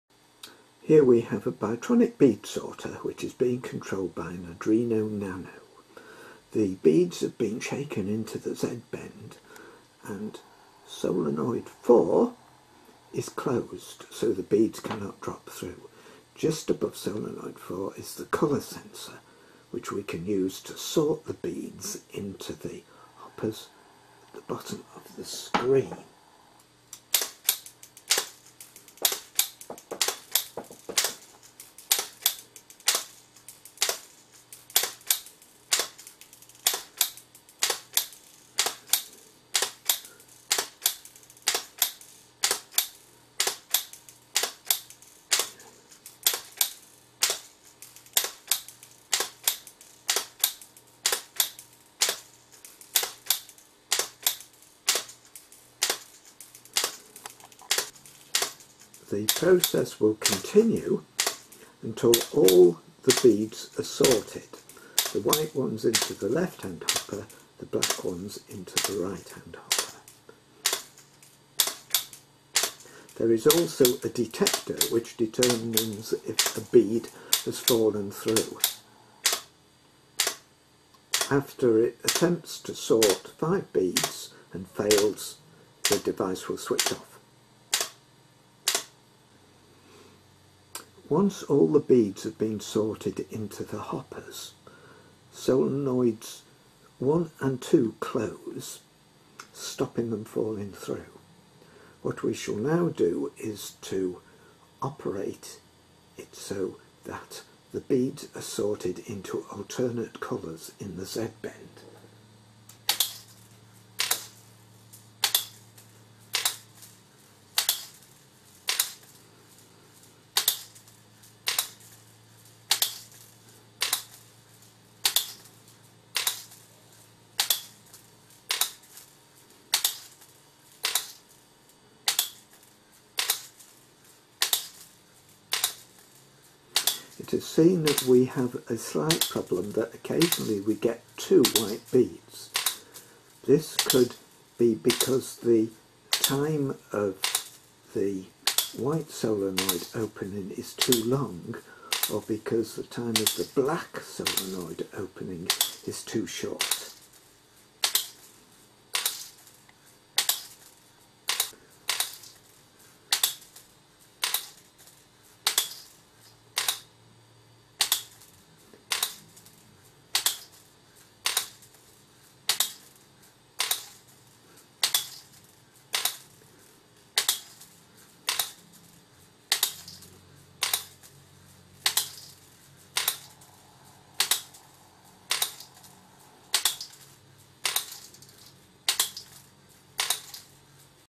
Bytronic Bead Sorter controlled by Arduino nano
this show an Arduino nano controlling a Bytronic Bead Sorter to
1. sort white and black beads into hoppers
2. sort from hoppers to obtain a white black white black etc pattern